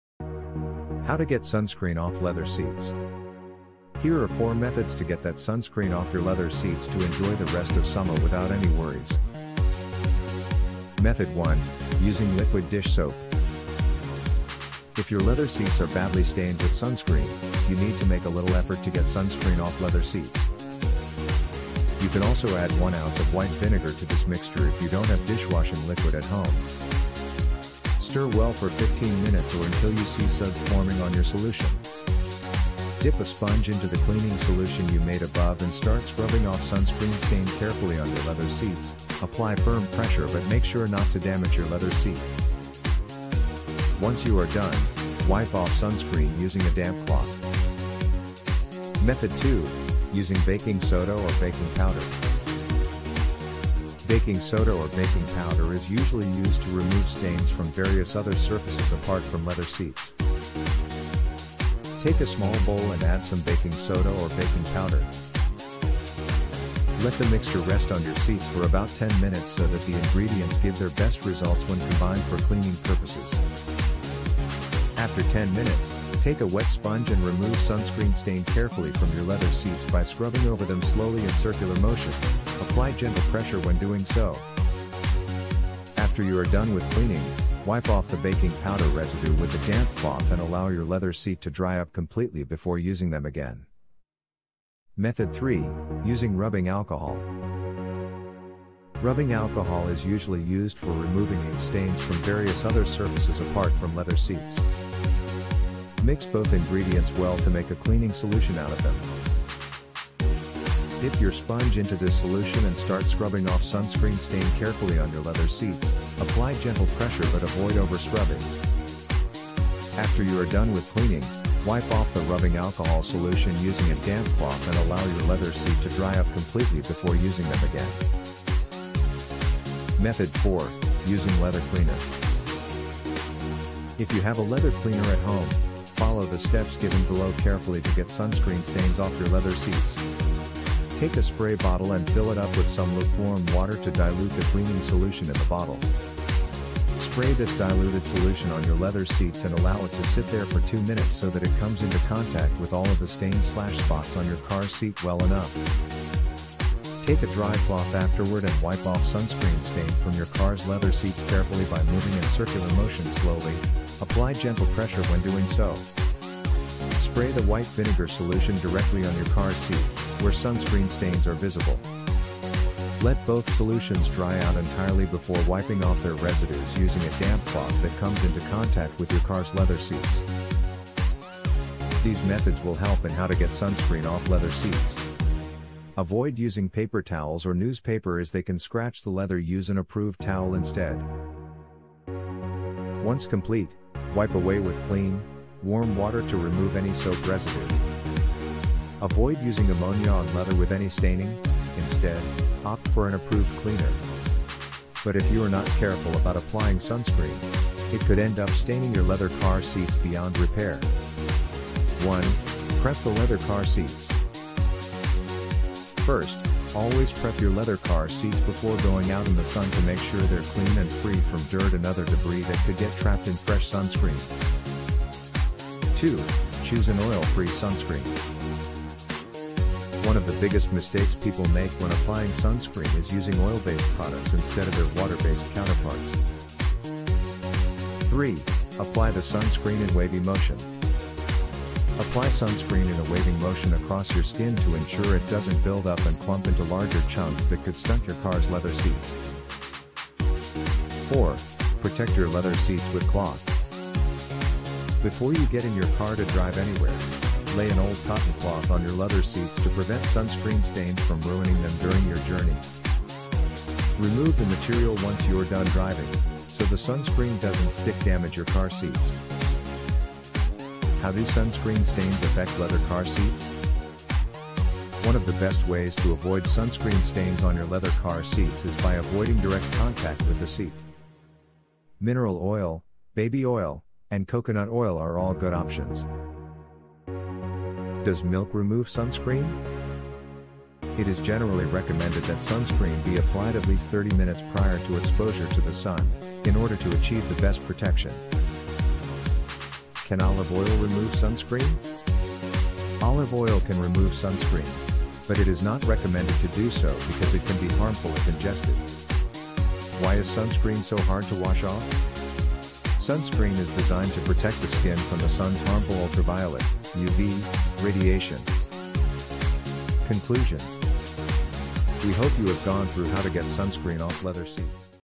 How to Get Sunscreen Off Leather Seats
If you've accidentally spilled sunscreen on your leather seats, don't panic! Removing it is easier than you might think.
First, use a soft cloth to wipe away any excess sunscreen from the leather surface. Then, apply a small amount of leather cleaner or rubbing alcohol onto the affected area and gently rub it in with a microfiber cloth.
Once the stain has been removed, use a clean damp cloth to wipe away any residue and let the leather air dry.
Remember to always spot-test any cleaning solution on an inconspicuous area before applying it to the whole surface.
Watch this video for more tips on how to keep your leather seats looking like new.